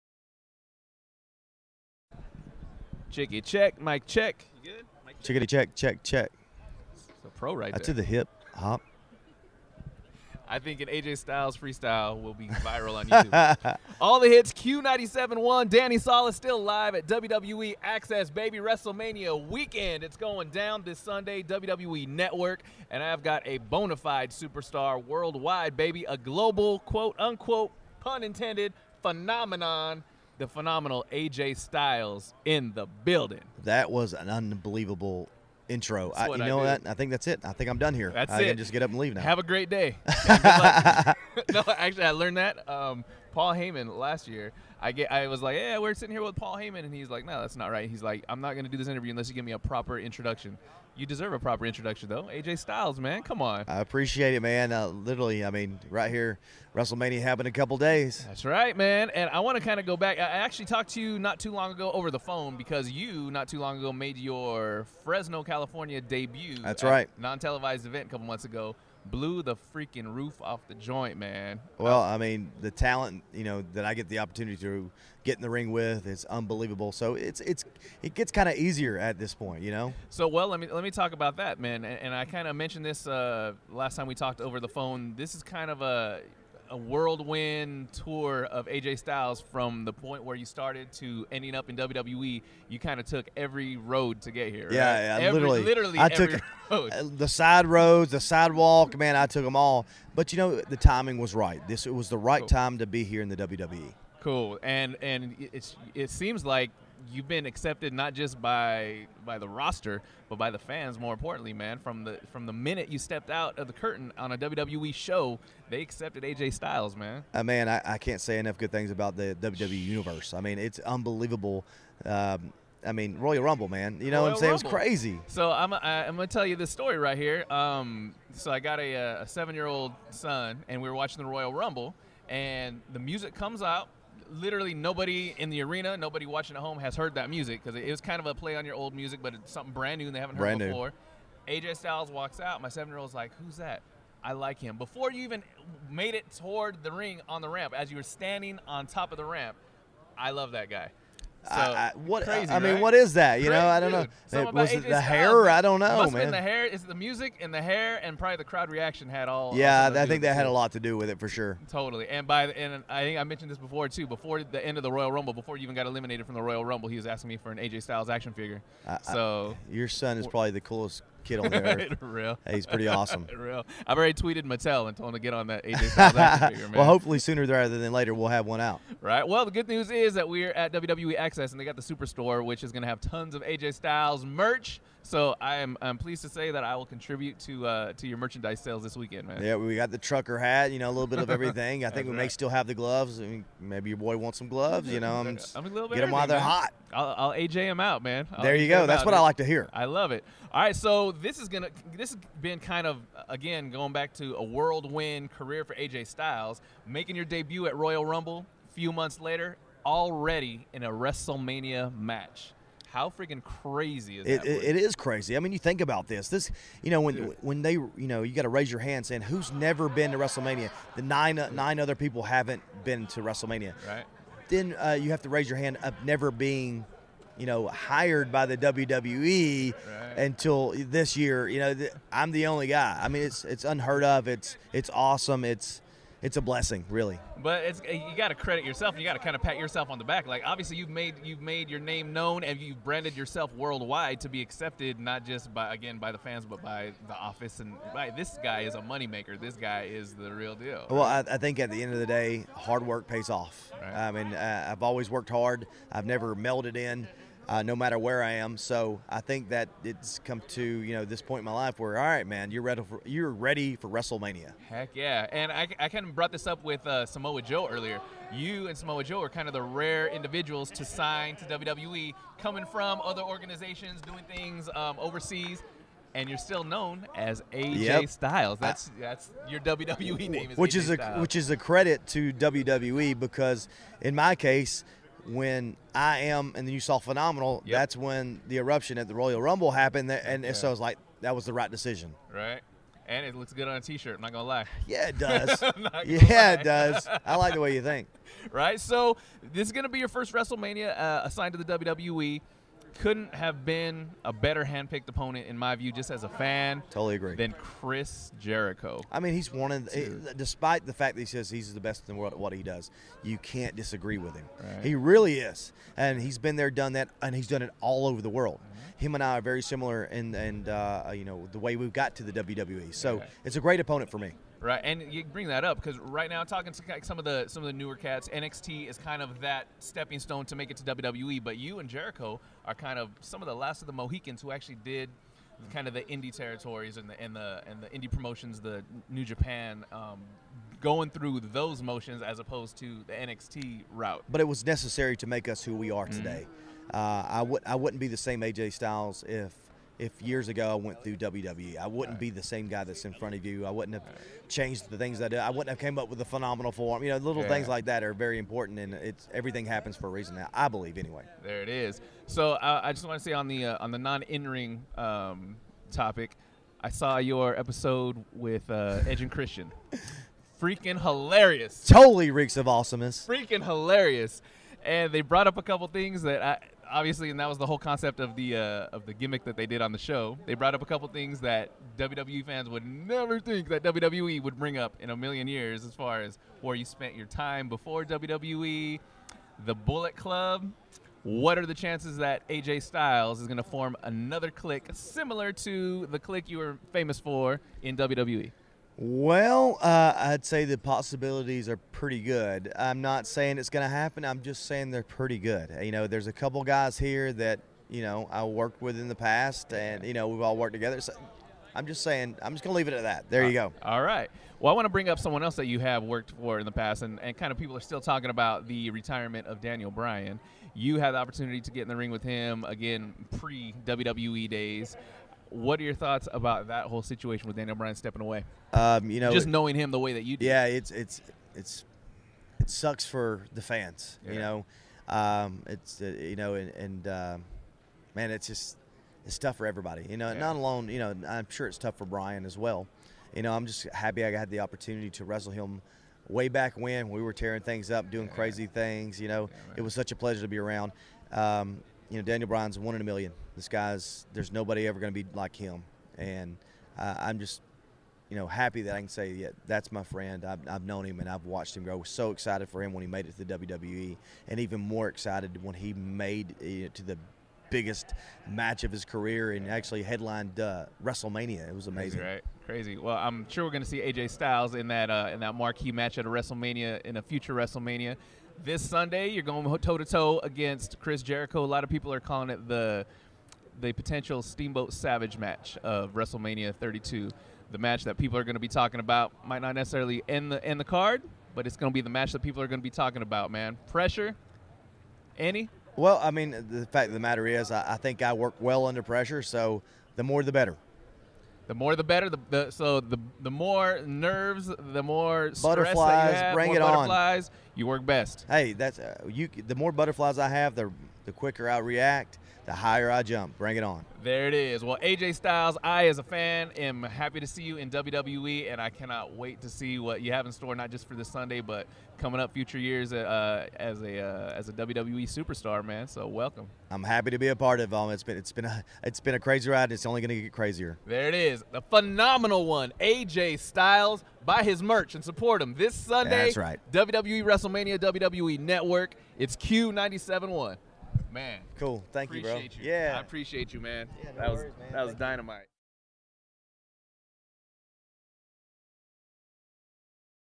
WWE Superstar AJ Styles at Wrestlemania!
4.1.2016 - Q97.1's Danny Salas was live in Dallas, Texas interviewing WWE Superstars before Wrestlemania 32! AJ Styles talked about the path he took to get to WWE, his debut at Royal Rumble, his merchandise, keeping his name in WWE, his match against Chris Jericho, the possibility of leading a stable in WWE, Daniel Bryan's retirement, and more.